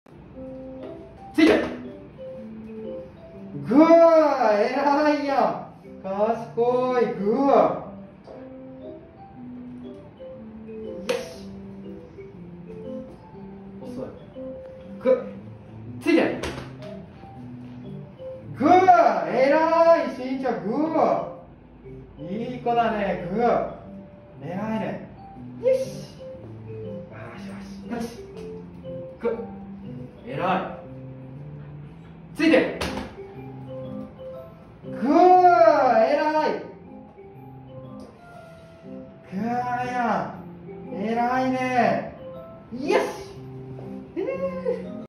柴犬の”シンバくん”と“ついて”の復習！
柴犬の”シンバくん”と“ついて”の復習をしています！
最初は“ついて”の指示を出すと違う場所で座ることがあったのでしっかりと誘導を使って人の左横につけるように練習☆徐々に誘導を減らしていきながら繰り返し練習していくと少しのサインで人の左横に”おすわり”が出来るようになりました(^_^)